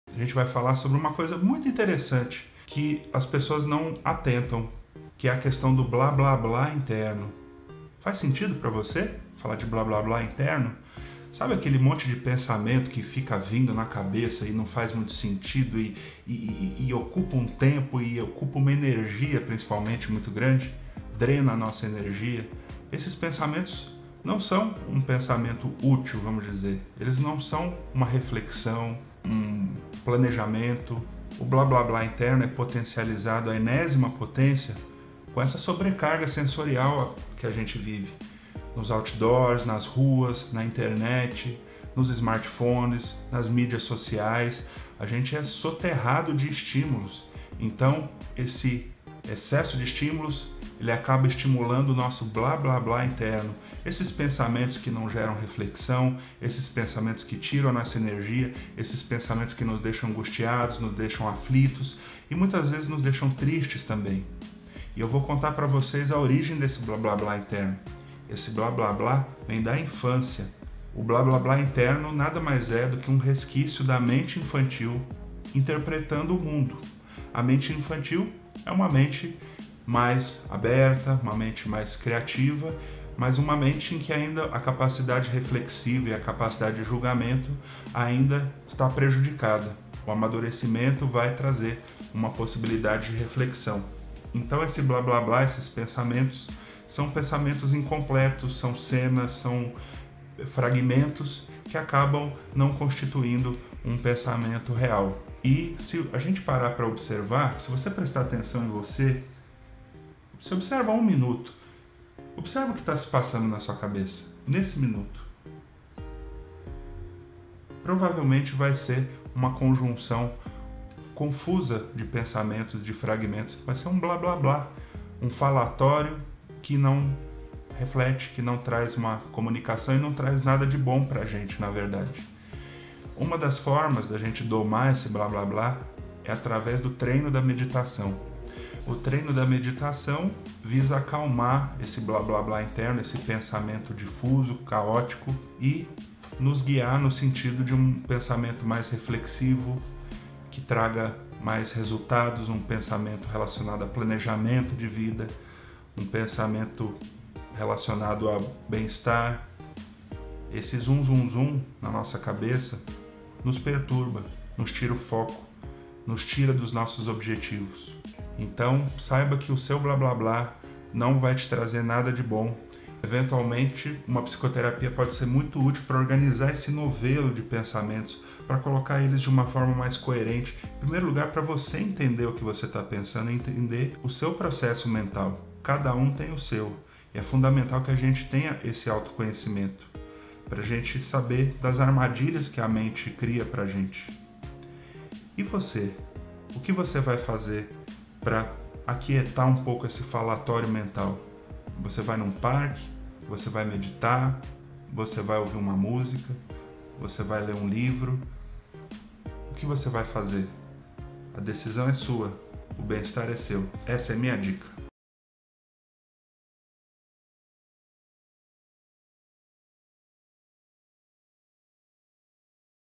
Falatório interno não traz nada de bom
Dr. Rodrigo Fonseca Martins Leite - psiquiatra do Instituto de Psiquiatria do HCFMUSP - Falatório interno não traz nada de bom.